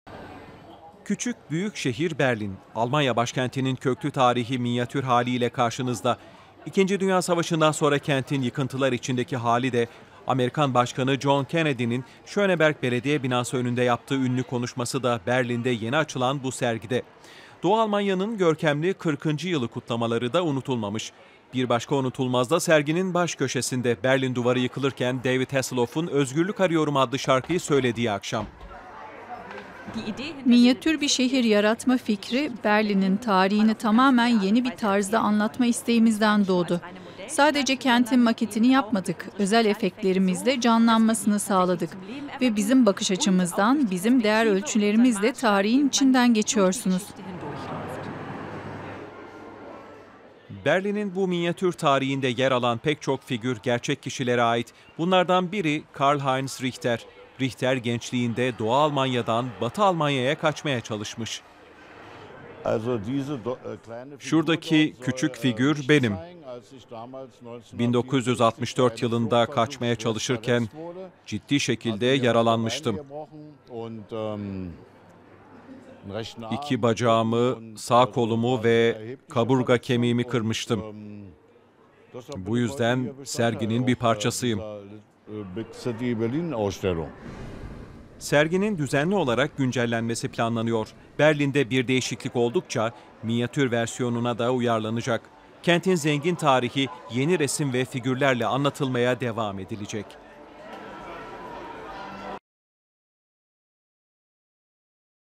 "Küçük Büyük Şehir Berlin" - DW Türkçe
İkinci Dünya Savaşı'nın yıkıntıları, Kennedy'nin ünlü konuşması ya da Doğu Almanya'nın 40'ncı yıl kutlamaları... Berlin'in tarihine bir de minyatür haliyle bakın.